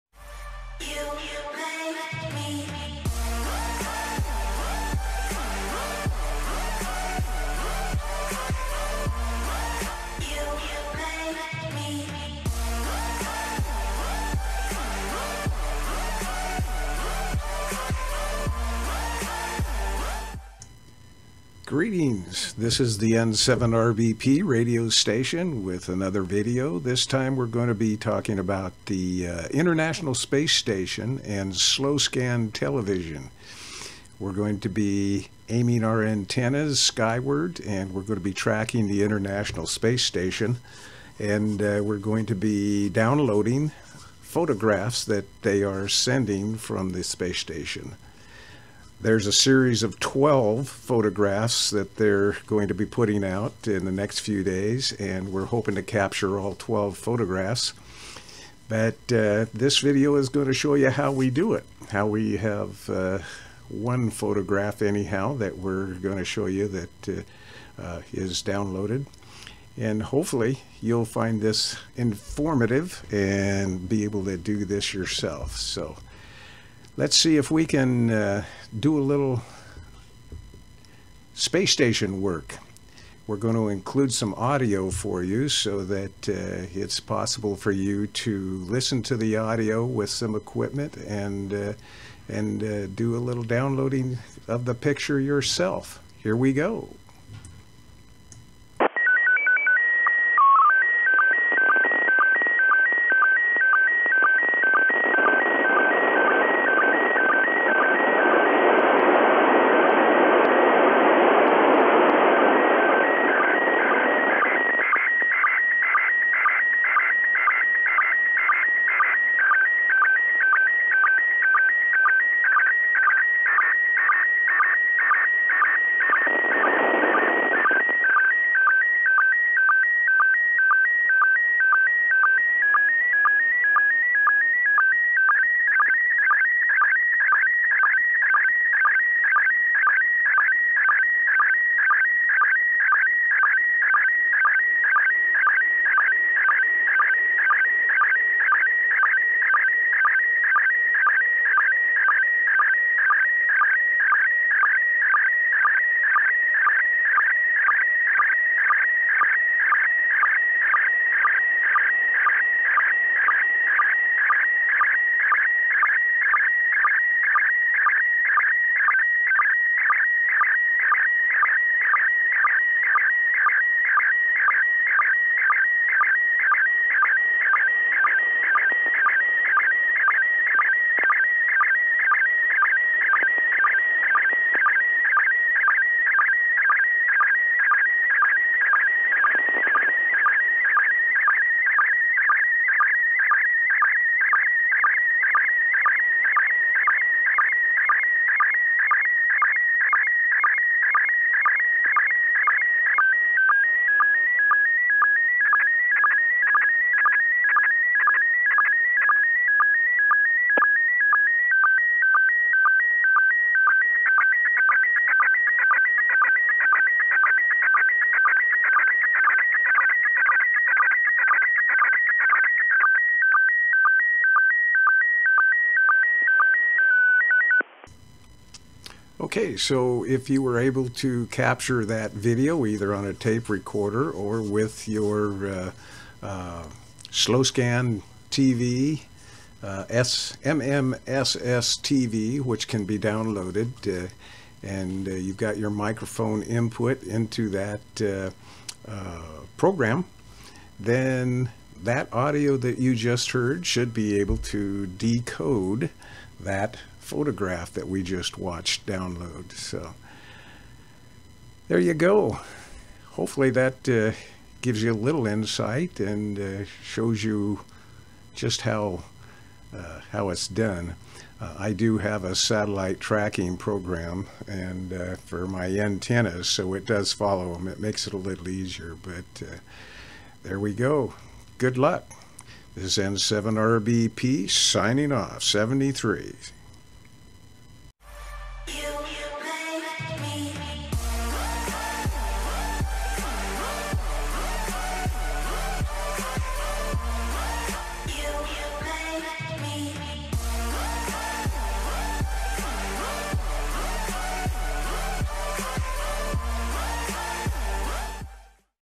Receiving Slow Scan TV from the International Space Station
Receiving Slow Scan TV (SSTV) from the International Space Station (ISS)